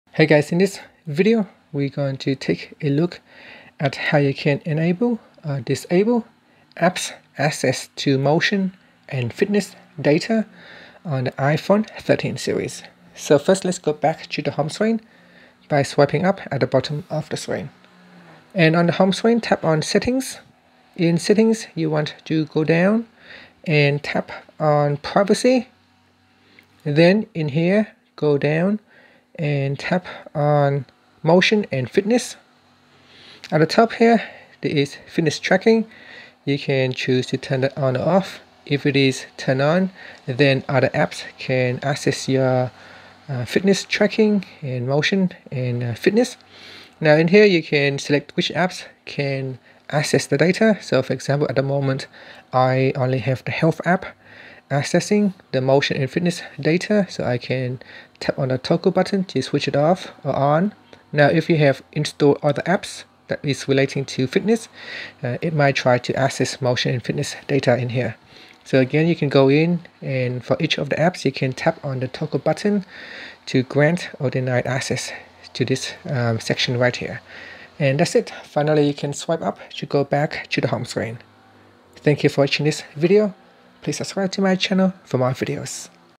iPhone 13/13 Pro: How to Enable/Disable Apps Access to Motion & Fitness Data
Learn how you can enable or disable apps access to motion and fitness data on the iPhone 13 / iPhone 13 Pro.